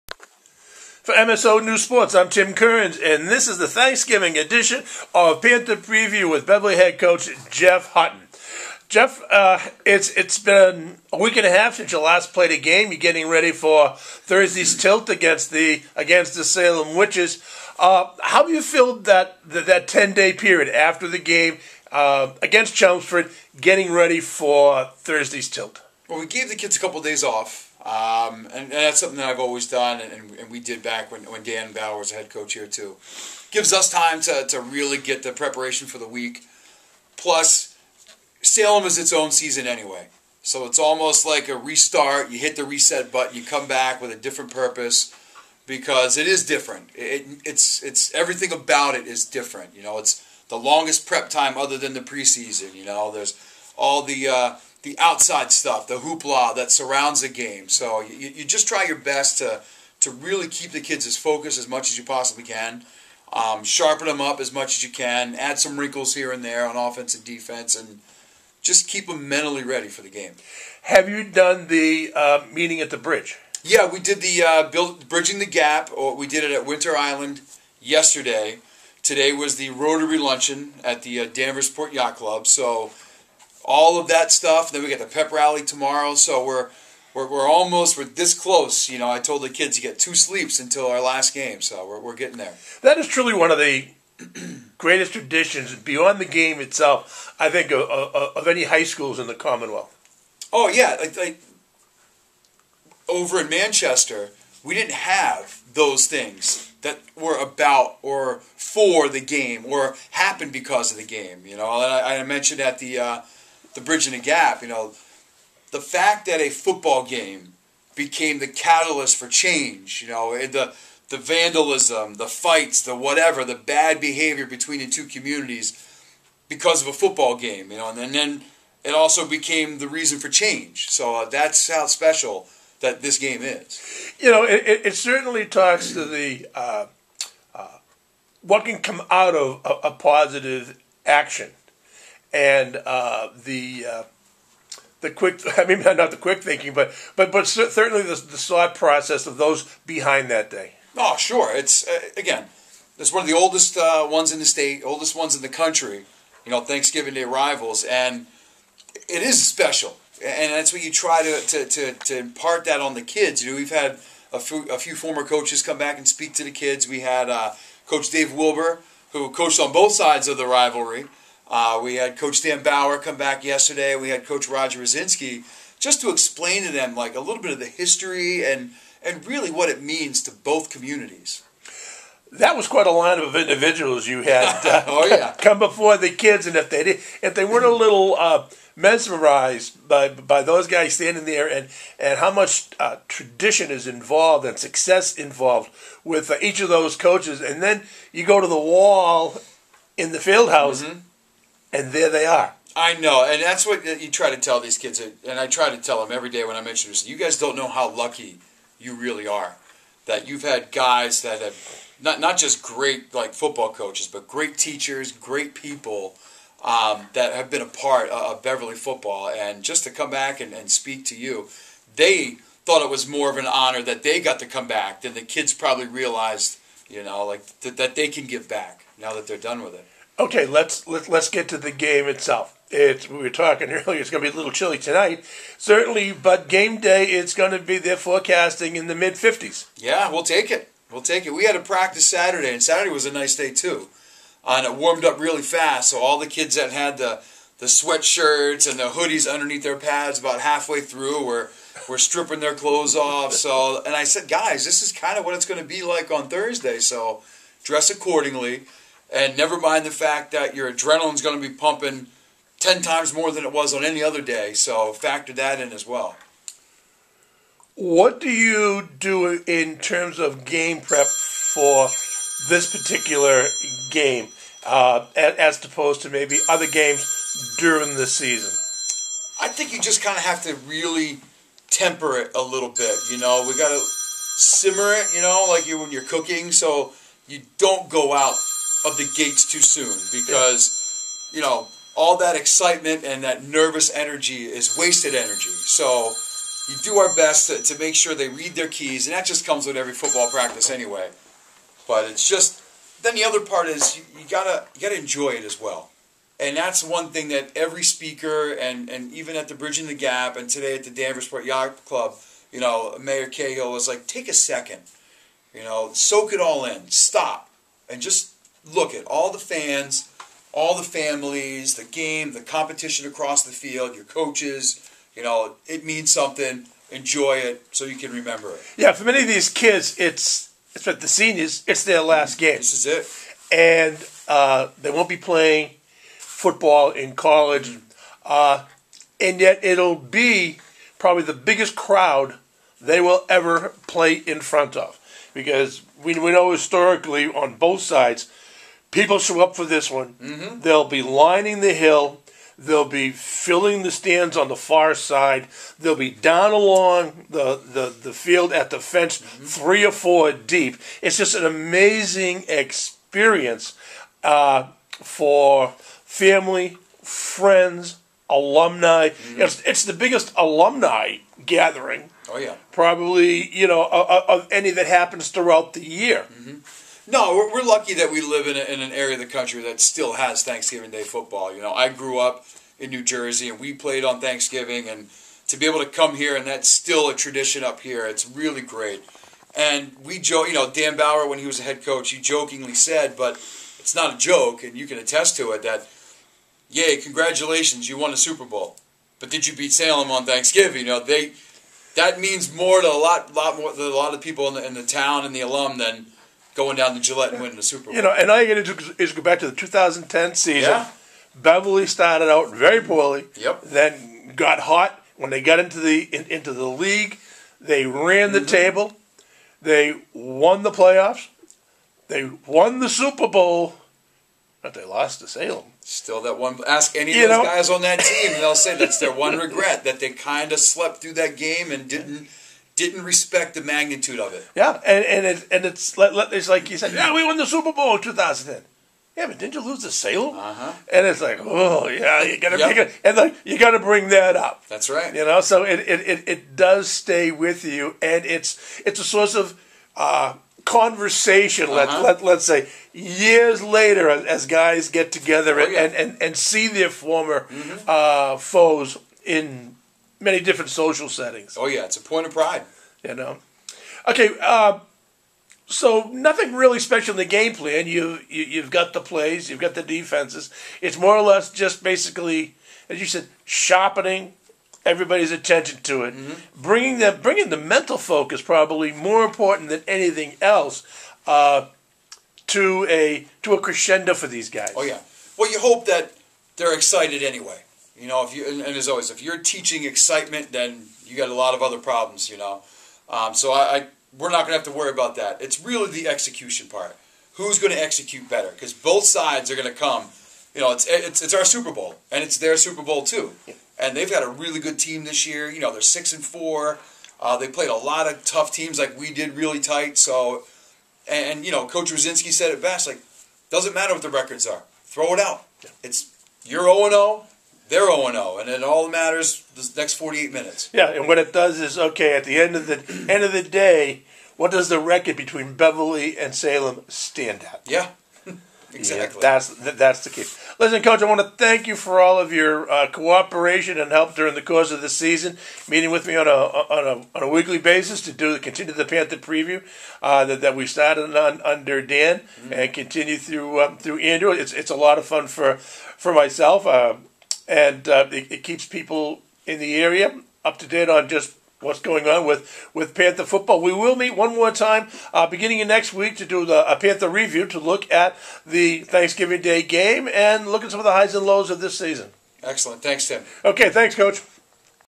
PantherPreview11232021
Coach Hutton talks turkey ahead of Thursday's traditional tilt versus Salem